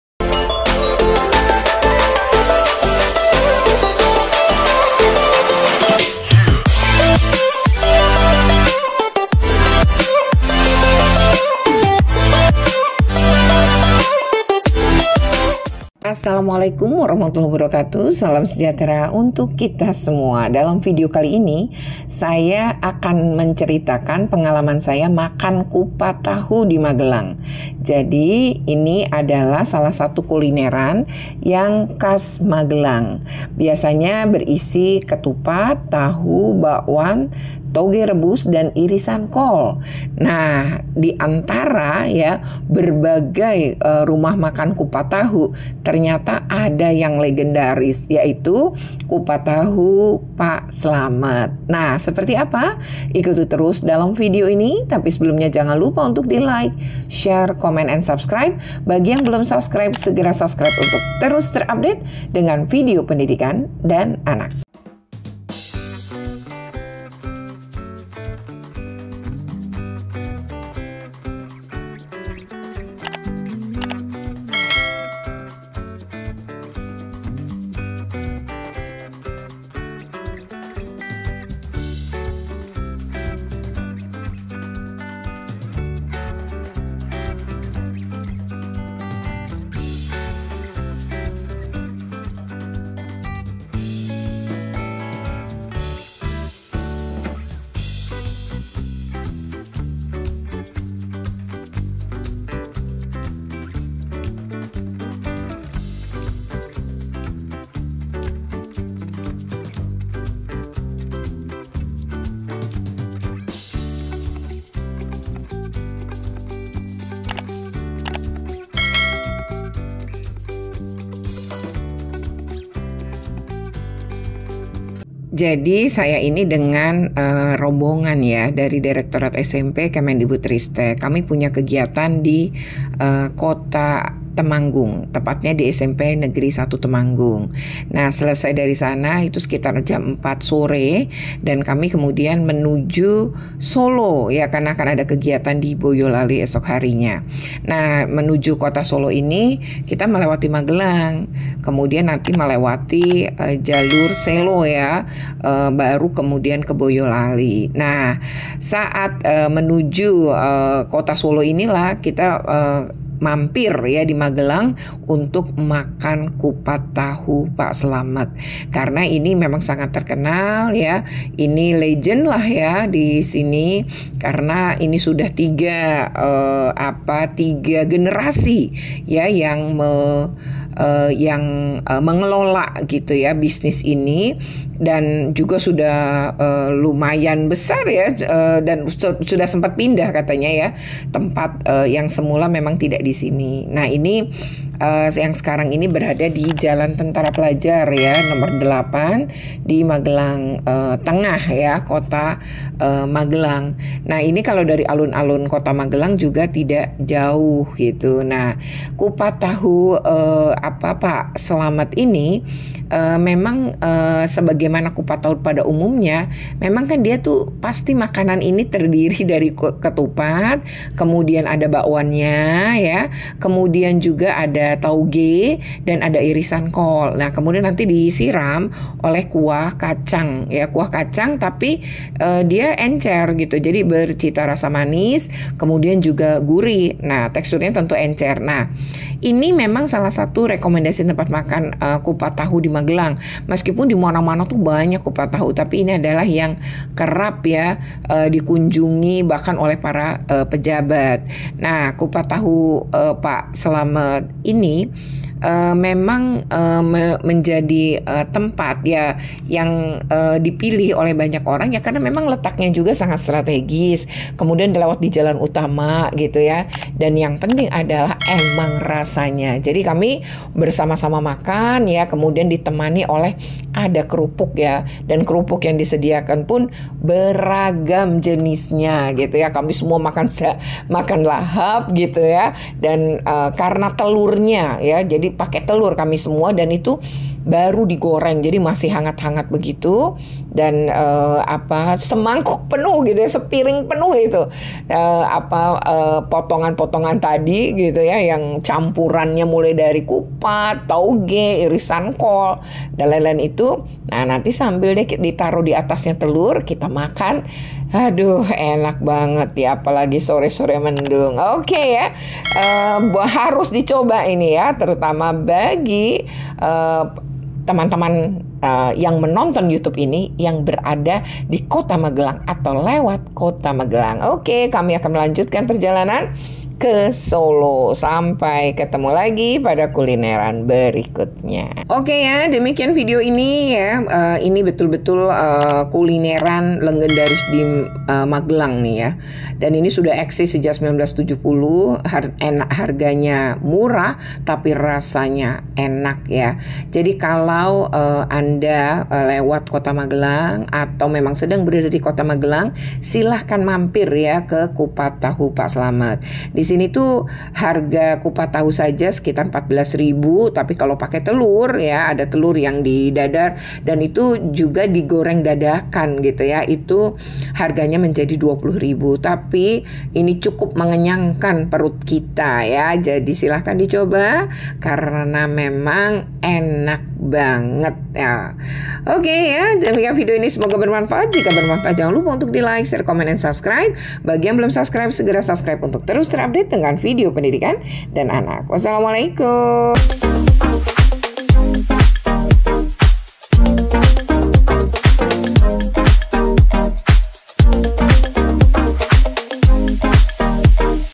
Kupat Tahu Pak Slamet, Kulineran legendaris diKota Magelang
Kupat tahu merupakan kuliner khas Magelang berisi kupat, tahu, bakwan, tauge rebus, dan irisan kol.Kemudian disiram kuah kacang bercita rasa manis dan gurih dengan tekstur encer.

Salah satu rekomendasi tempat makan kupat tahu di Magelang yang wajb dicoba adalah Kupat Tahu Pak Slamet yang merupakan salah satu kuliner legendaris di Magelang, karena telah dibuka pertama kali pada 1970-an.

Seperti apa keseruannya, simak dalam link video berikut ini.